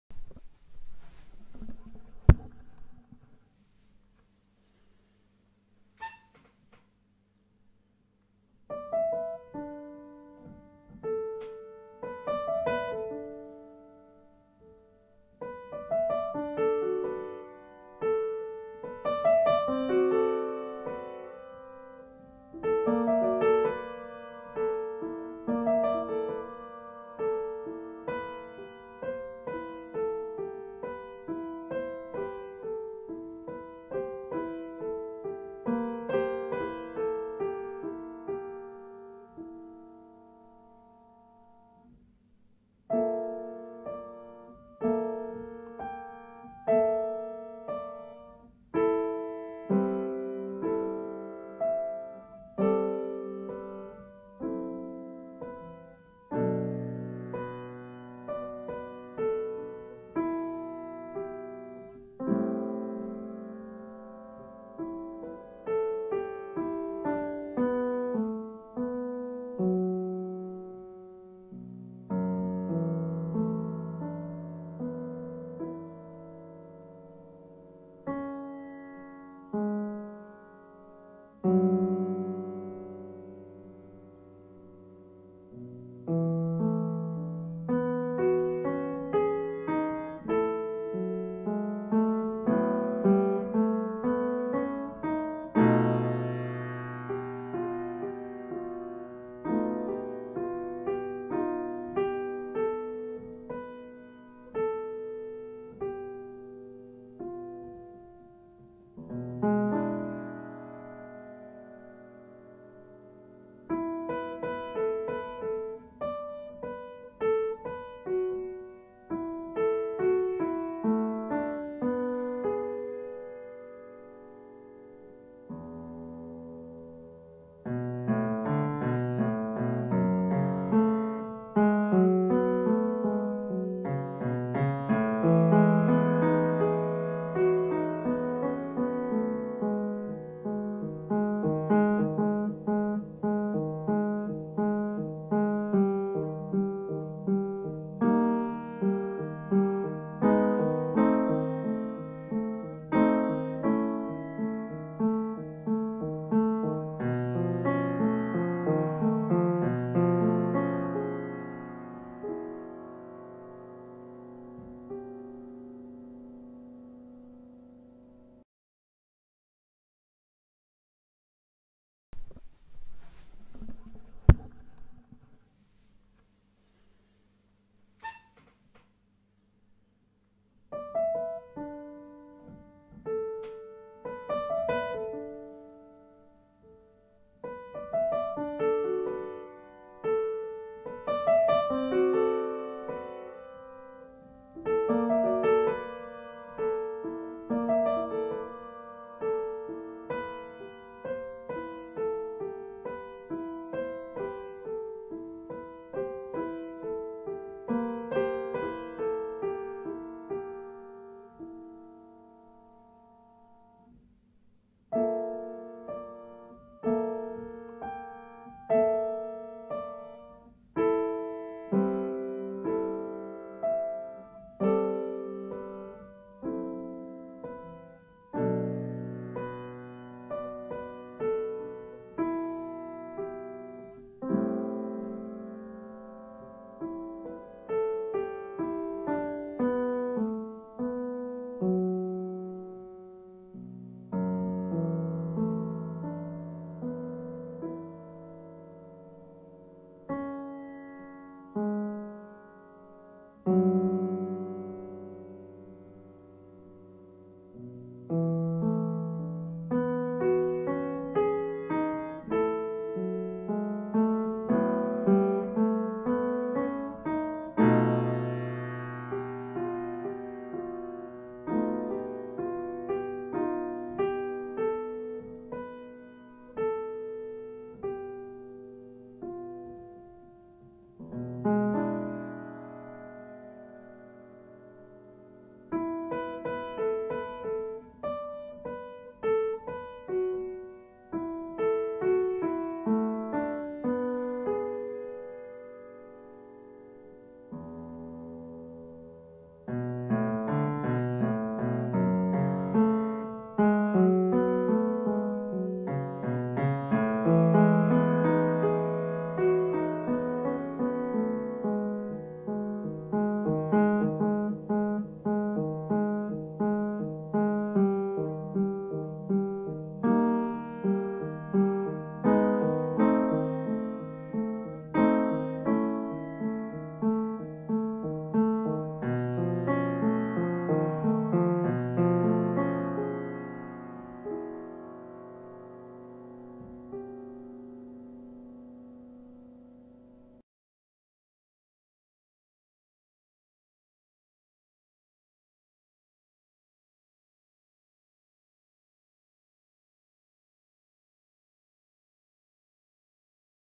Ibrahim Jawabreh (2) - Palestinian Painter
IBRAHIM JAWABREH is a Ramallah-based visual artist born in Aroub refugee camp. Video and piano music by Eitan Altman